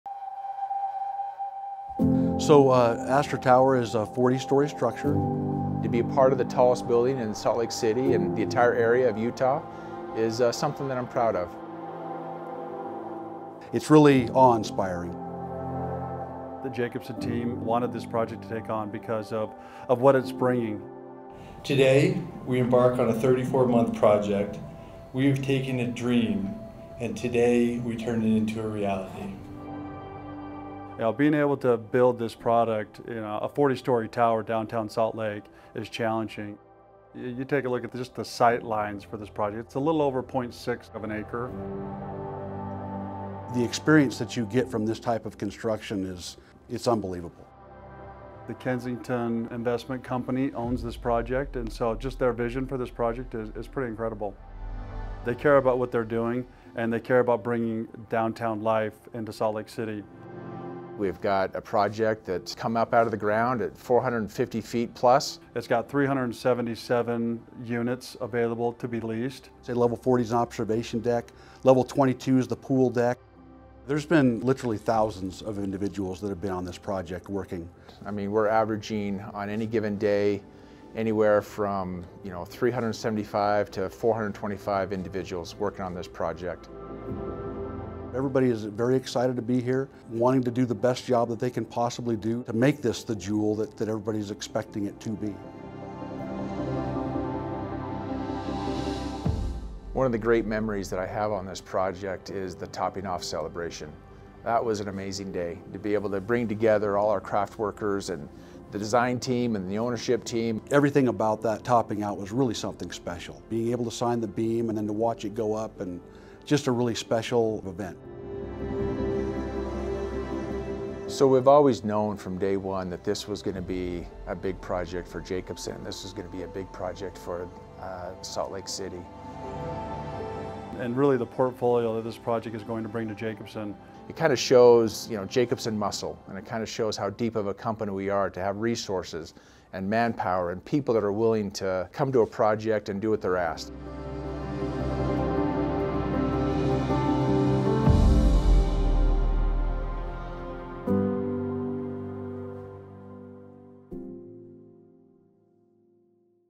Astra Tower: How Jacobsen Construction Took Utah to New Heights
Astra Tower, a 450-foot luxury apartment high-rise in downtown Salt Lake City, is Utah's new tallest building. Comprised of 40 stories, Astra Tower includes 377 studio, one-, two- and three-bedroom units, as well as two levels of penthouse suites. The high-rise welcomed its first residents in August 2024. Its best-in-class amenities include an urban park atop the five-level parking structure, a full-service fitness center with multipurpose studio and a clubhouse with a chef kitchen and movie theater. Astra Tower is a LEED Gold (official certification pending) building that was designed and constructed to address environmental issues prevalent in northern Utah, like air quality and water conservation.

With its prominent place on the skyline, Astra Tower stands out as an instant Salt Lake City icon and a stirring showcase of Utah builders’ growing capacity to build to taller-than-ever heights on tight jobsites in busy urban locations. Using as many as 400-plus construction professionals per day, Astra Tower was a show of high-rise construction "muscle" for Jacobsen Construction, its trade partners and the state's construction industry as a whole, Project Manager Doug Carley says. Watch this video to learn more from Doug and other project leaders about what it was like to build this transformative high-rise from the ground up!